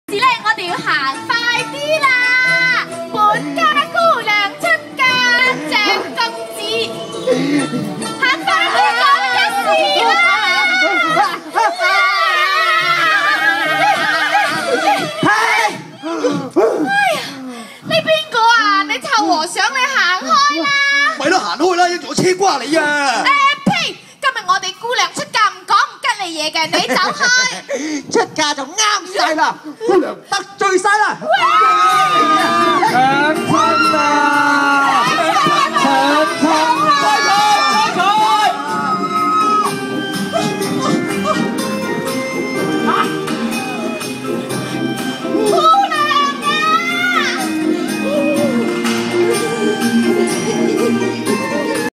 2012社區文化大使《濟公傳奇之人不可以貌相》@九龍寨城公園演出(2)
此片拍於22/4/2012@九龍寨城公園。劇場工作室2012社區文化大使《創出香港神話2~濟公傳奇之人不可以貌相》通過16場社區巡迴的週未假日演出(15/4-12/5/2012)，以輕鬆手法糅合熱鬧唱誦、舞蹈、音樂及布偶等戲劇元素，演繹出濟公的神話故事，令普羅大眾接觸多點文化藝術。並希望發展成為一系列神祇的伸延劇場活動。

編劇導演余翰廷 + 作曲編曲何俊傑 + 填詞陳文剛 + 編舞黃志榮 + 佈景服裝設計章佩佩 + 布偶操控指導余健生
演員 鄭國偉+余健生+尹偉程+胡俊謙+陳郁憲+鍾詠詩+鄭佩嘉+賴慰玲
舞台監督 李浩賢+馮舒凝、執行舞台監督Mad Music梁金達+葉志堅、助理舞台監督 陳漢光+李家賢
監製廖淑芬、計劃統籌劉穎妍